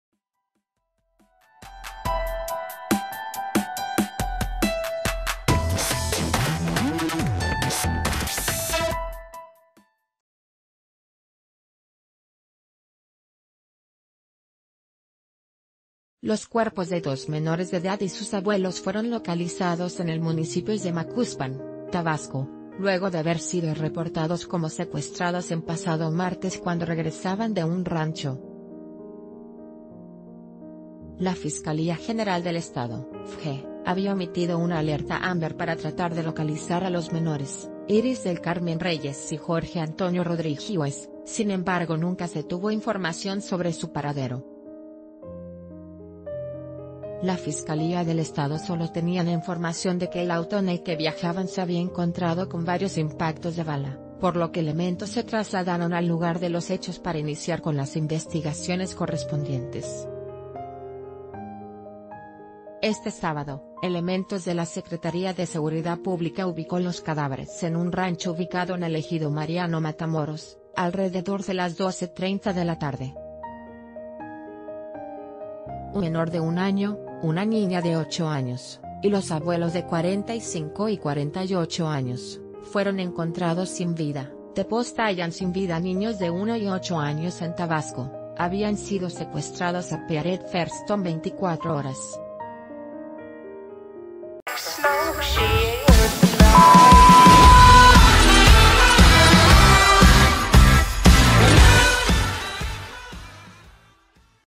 Hallan sin vida a niños de uno y ocho años en Tabasco; habían sido secuestrados
Los cuerpos de dos menores de edad y sus abuelos fueron localizados en el municipios de Macuspan, Tabasco, luego de haber sido reportados como secuestrados en pasado martes cuando regresaban de un rancho. La Fiscalía General del Estado (FGE), había emitido una Alerta Amber para tratar de localizara ...